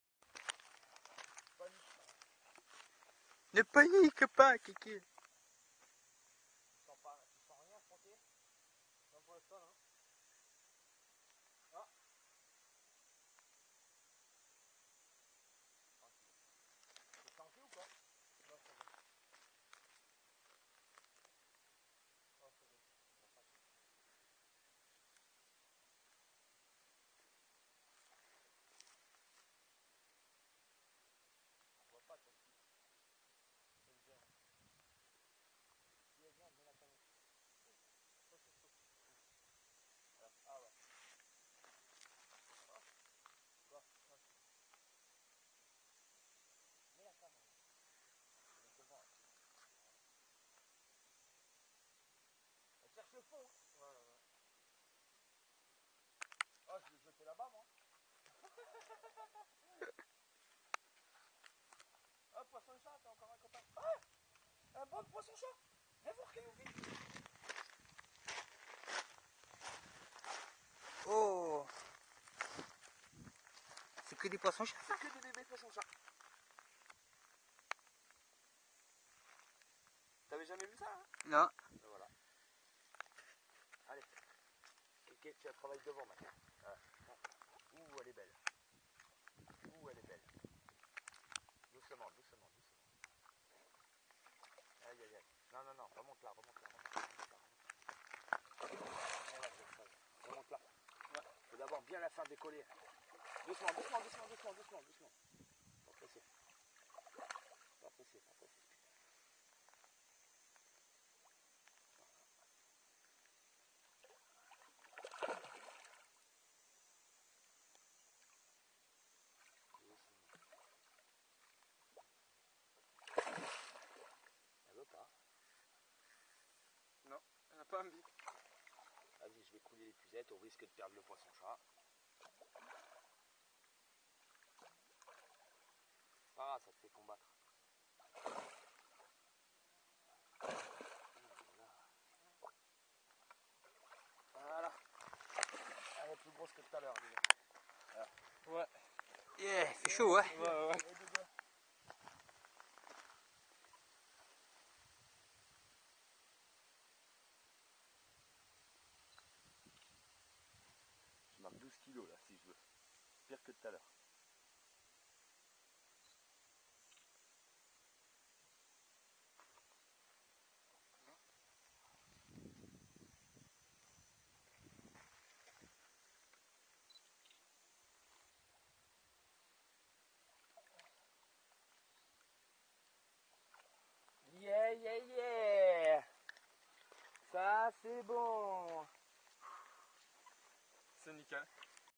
peche a la carpe dans le gard et combat d'une belle carpe commune !!
belle carpe commune prise dans le gard a la bille signal !!!